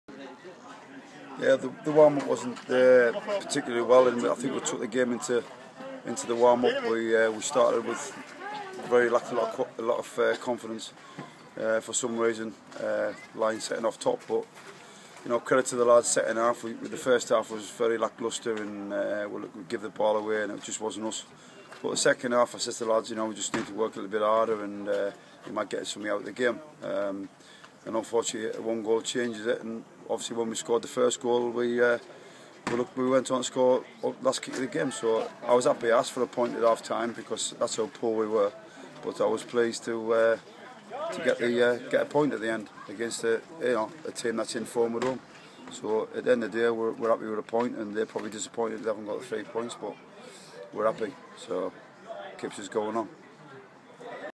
ARCHIVE - Post match interview from Manager Andy Liddell - Knaresborough V Cleethorpes Town
Post match interview from Manager Andy Liddell - Knarsborough v Cleethorpes Town 17th November